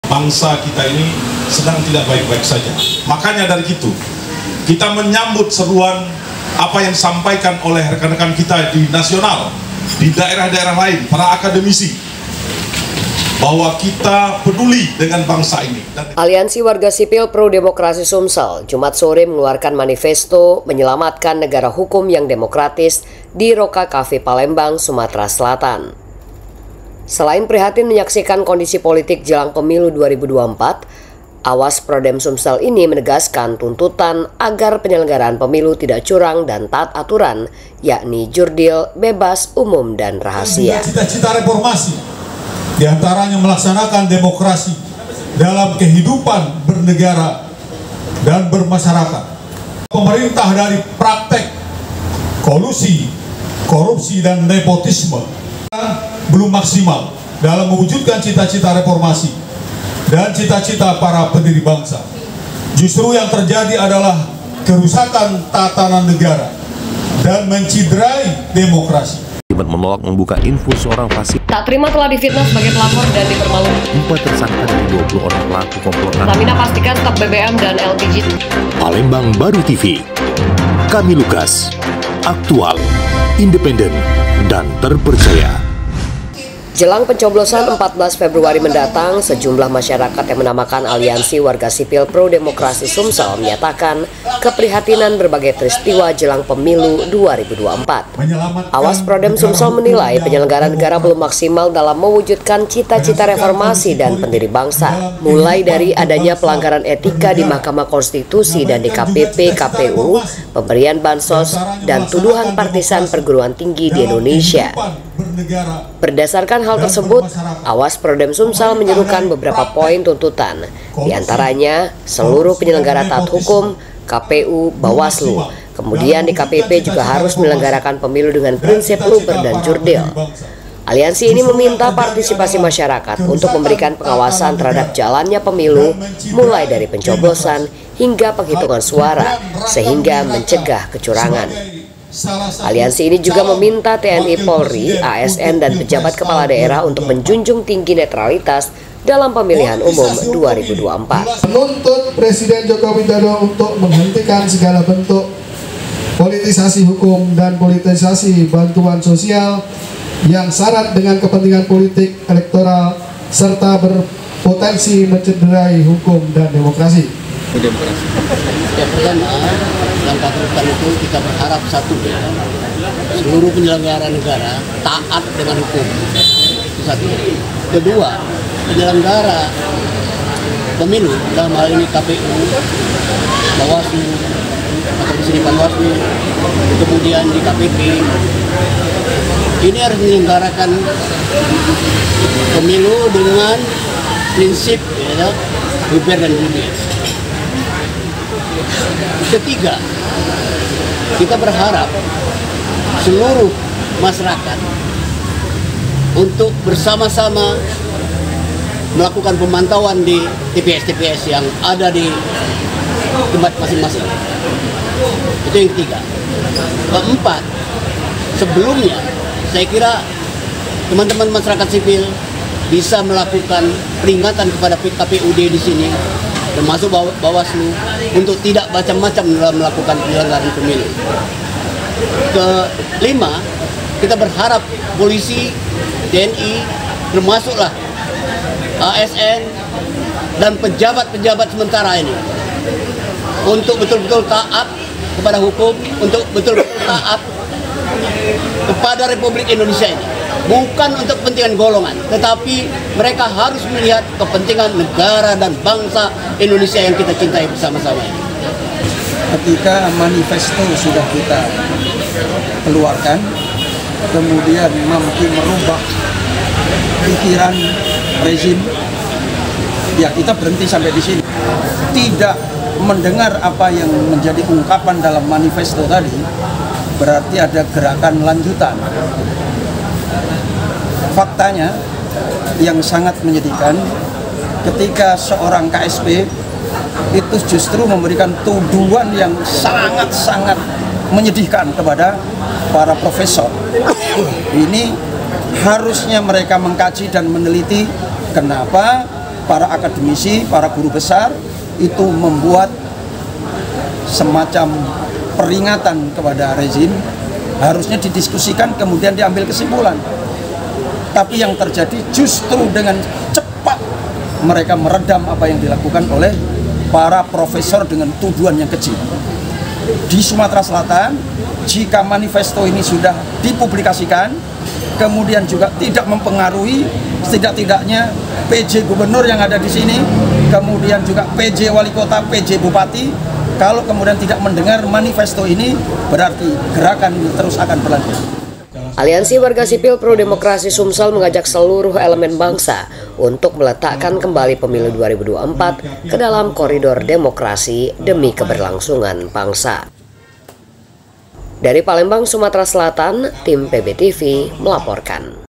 Aliansi Warga Sipil Pro Demokrasi Sumsel Desak Penyelenggara Pemilu Tak Curang
Palembang - Sejumlah tokoh dari berbagai kalangan di Sumatera Selatan, termasuk pengacara, ulama, aktivis, akademisi, dan masyarakat umum, yang berada di bawah payung Aliansi Warga Sipil Pro Demokrasi (Awas Prodem) Sumsel, menggelar deklarasi Manifesto Menyelamatkan Negara Hukum yang Demokratis.

Acara tersebut digelar di Cafe Roca, Jalan Demang Lebar Daun, Palembang, pada Jumat (9/2/2024).

"Manifesto tersebut berisi tujuh sikap Awas Prodem Sumsel," ungkap juru bicara Awas Prodem Sumsel, Tarech Rasyid, dalam pernyataannya.

Dia menambahkan, gerakan tersebut muncul sebagai respons terhadap kegelisahan di kalangan guru besar dan civitas akademika di Indonesia. "Gerakan ini didasarkan pada kajian luas dan bukan sekadar keresahan semata," ungkapnya.

Dalam acara tersebut, hadir tokoh-tokoh seperti pengacara Darmadi Jufri, Idasril Firdaus Tanjung, aktivis Sumsel Lagan, Febuar Rahman, budayawan Erwan Suryanegara, ulama Ustad Umar Said, dan akademisi Tarech Rasyid.

Salah satu poin manifesto yang dibacakan adalah menolak segala bentuk penyelenggara negara yang mengingkari etika, moral, serta nilai-nilai luhur Pancasila dan UUD 1945.

"Jika rezim tidak mendengarkan manifesto ini, gerakan ini akan terus berlanjut," tambah Ulama Ustad Umar Said.

Manifesto Menyelamatkan Negara Hukum yang Demokratis tersebut berisi 7 sikap Awas Prodem Sumsel:

Pertama : Menolak semua para penyelenggara negara yang mengingkari etika, moral, nilai- nilai luhur Pancasila dan UUD 1945 di dalam kehidupan berbangsa dan bernegara,karena dapat merusak tatanan negara dan mencederai demokrasi sehingga terjadi pelemahan Negara hukum yang demokratis.

Kedua : Mendesak Presiden Joko Widodo untuk menegakkan kewibawaan dan martabat Presiden sebagai negarawan, sehingga presiden sebagai penyelenggara negara mampu mewujudkan cita-cita reformasi dan cita-cita para pendiri bangsa untuk membangun negara hukum yang demokrastis.
Ketiga : Menuntut Presiden Joko Widodo untuk menghentikan segala bentuk politisasi hukum dan politisasi bantuan sosial yang sarat dengan kepentingan politik elektroral serta berpotensi menciderai hukum dan demokrasi.
Keempat : Menuntut lembaga penyelenggara Pemilu,KPU/KPUD, Bawaslu, DKPP untuk bekerja profesional, tranparan, dan bersikap netral untuk menjaga demokrasi dengan mengedepankan azas LUBER (Langsung,Umum, Bebas, dan Rahasia) dan JURDIL (Jujur dan Adil) berdasarkan Pancasila dan UUD 1945.
Kelima : Menuntut TNI, Polri,Menteri,Pj Gubernur,Pj Bupati,Pj Walikota,Aparat Sipil Negara (ASN), dan Kepala Desa diseluruh Indonesia, agar bersilkap netral dan tidak cawe-cawe dalam Pemilu 2024 sebagai upaya membangun negara hukum yang demokratis.

Keenam : Menuntut para penyelengara negara untuk menghentikan segala tidakan intimidasi dan mengintervensi para Rektor dan Sivitas Akademika atau universitas/kampus yang melanggar prinsip-prinsip demokrasi, hukum dan kebebasan akademik.

Ketujuh : Menghimbau masyarakat Indonesia untuk terlibat dan mengawal penyelenggaraan Pemilu agar tidak terjadi kecurangan dan memastikan bahwa Pemilu 2024 dapat menghasilkan pemerintahan yang memiliki legitimasi kuat yang berbasis suara rakyat.